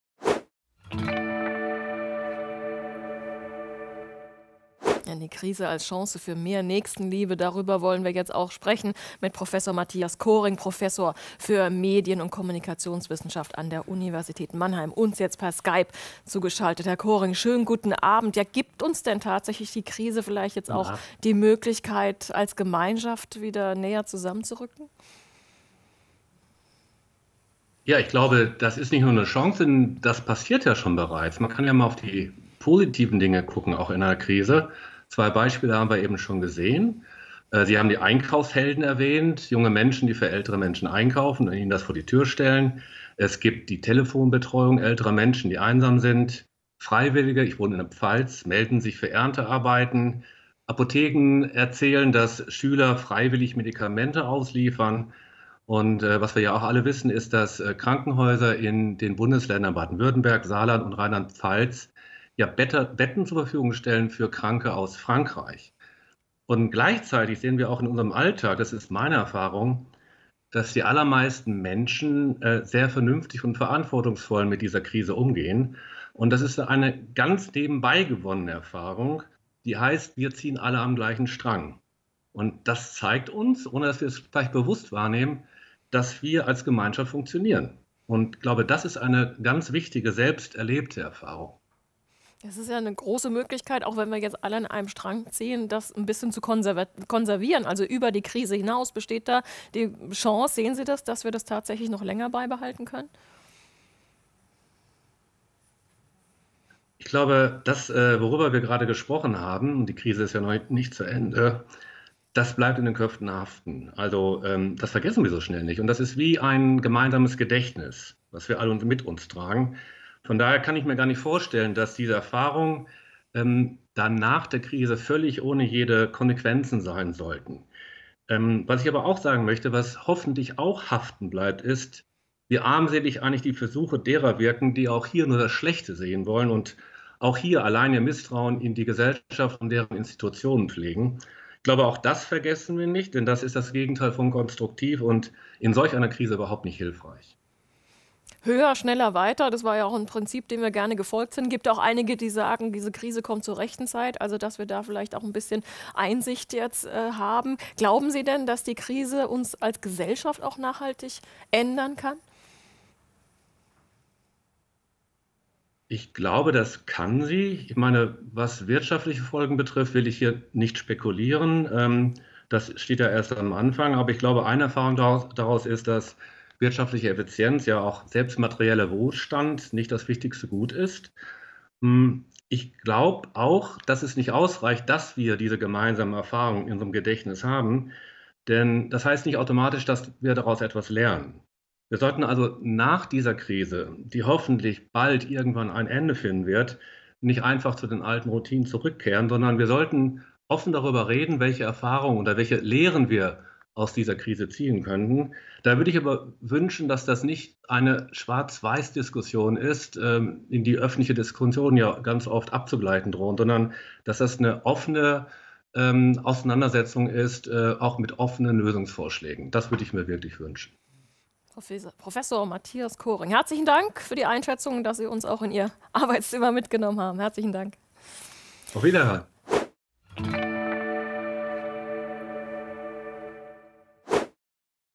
Das macht Corona mit dem Menschen | RON TV
Verhalten in der Krise: Im Studio ist der Medien- und Kommunikationswissenschaftler Prof. Matthias Kohring